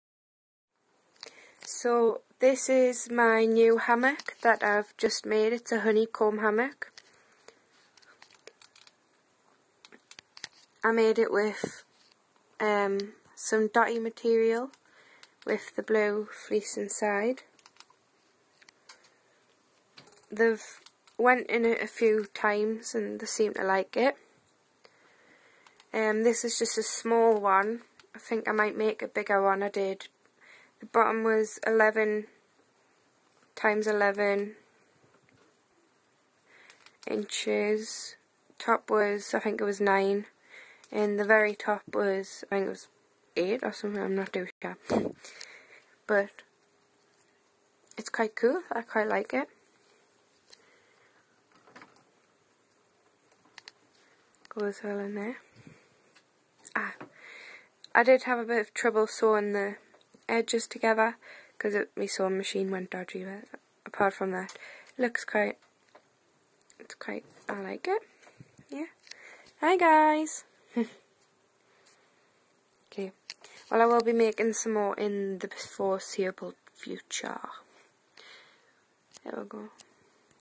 Handmade Honey comb hammock
Decided to make my own hammock :D Followed the instructions of SmallPetChannel so thankyou :D